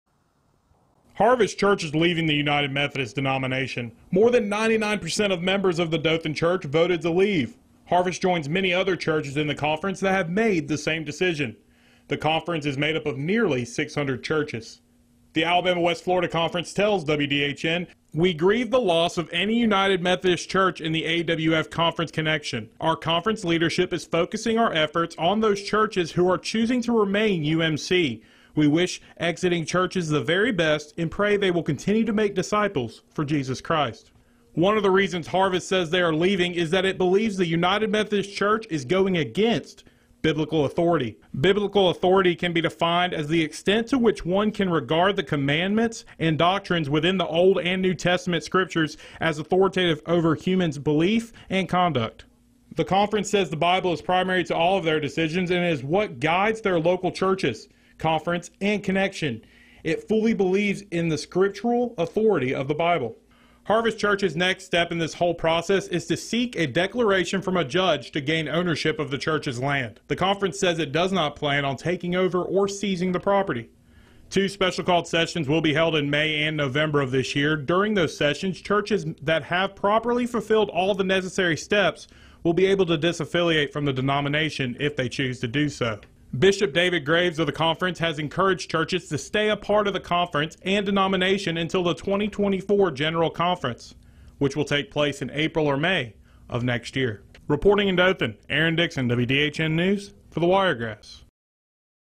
United Methodist Conference responds to the exodus of churches
The United Methodist Church is responding to the exodus of churches after Harvest Church in Dothan chose to leave the denomination with an astounding 99% vote.